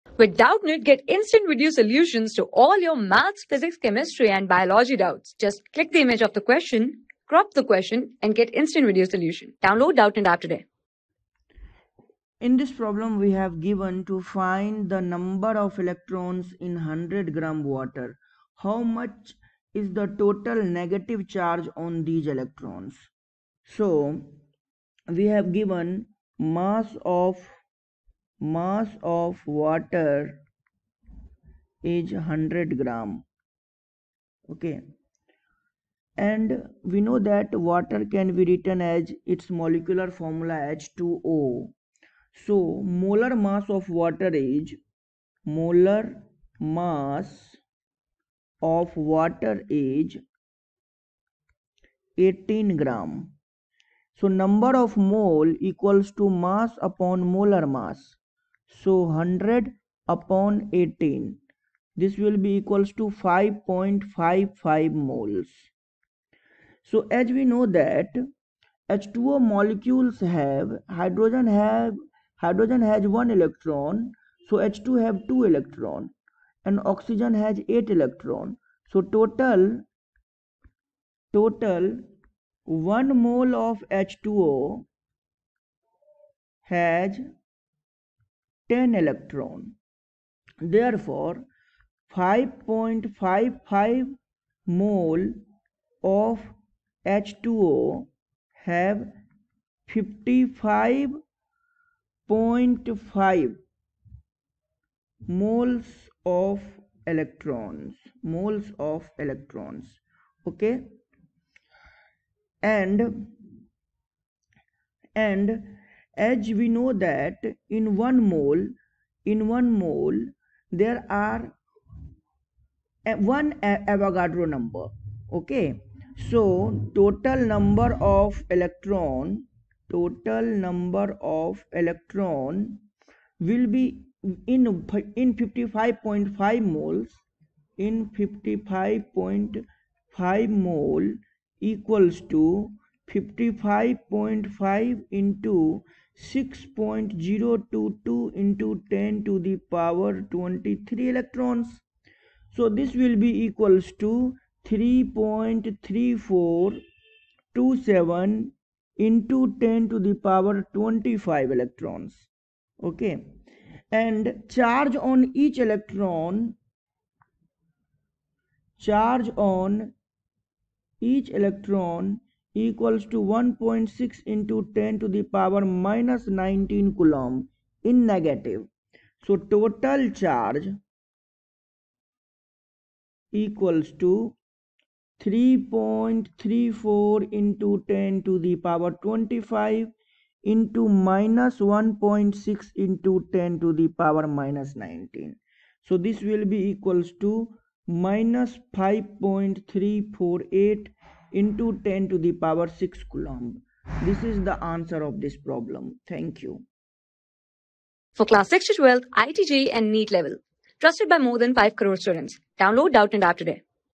Estimate the number of electrons in 100 g water. How much is the total negative charge on these ...
Estimate the number of electrons in 100 g water. How much is the total negative charge on these electrons?
Class: 12
Subject: PHYSICS
Chapter: ELECTRIC FIELD AND POTENTIAL
Board:IIT JEE

👉 Have Any Query? Ask Us.